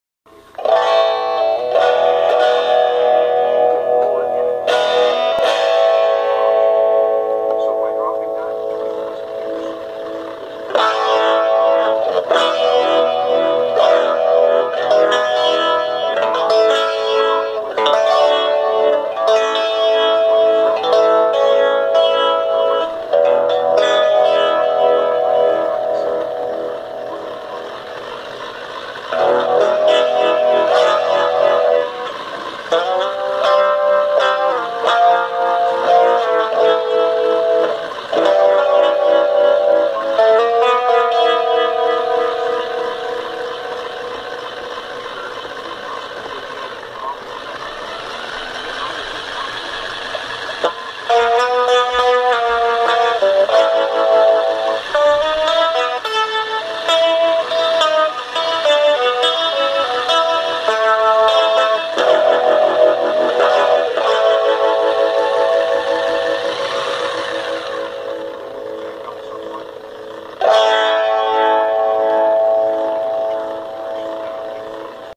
Rotating speaker test
Testing a little rotating speaker with guitar